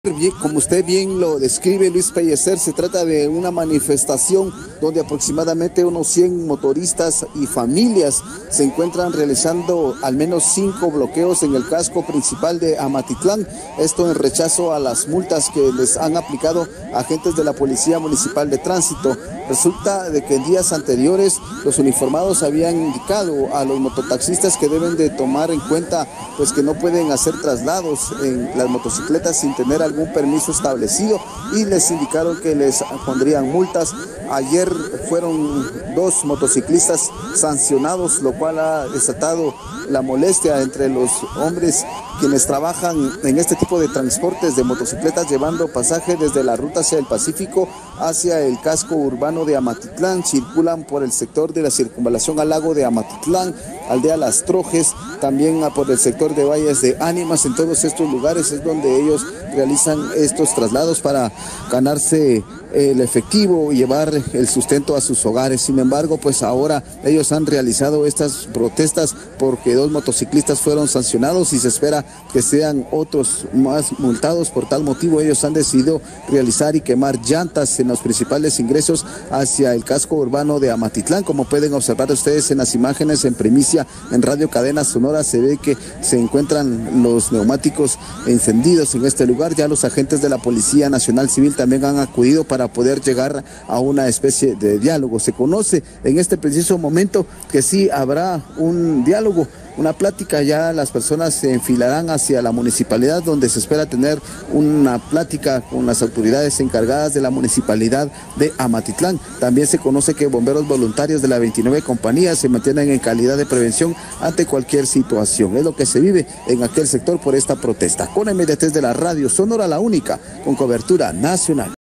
Motaxistas bloquean y queman llantas en Amatitlán (VIDEO)
Mototaxistas han bloqueado el paso en Amatitlán.